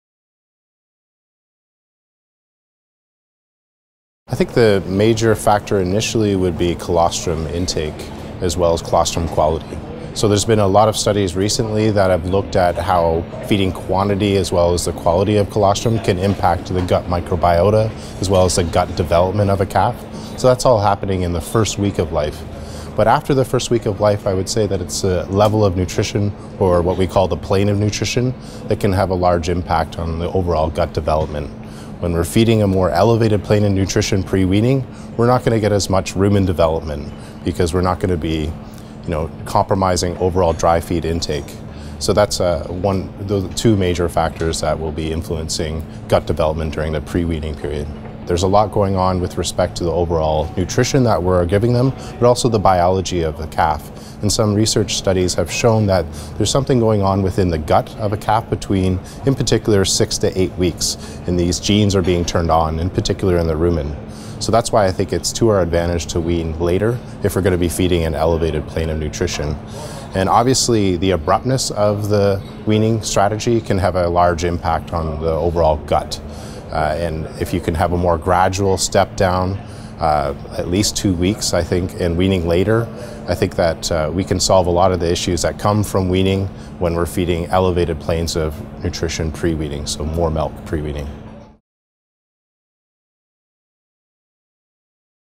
Dietary factors to optimize pre- and post-weaning gastrointestinal development of calves
Dr Michael Steele explains the importance of colostrum and nutritional plane in setting up positive gastrointestinal health of neonatal calves, as well as weaning strategies that support gut health.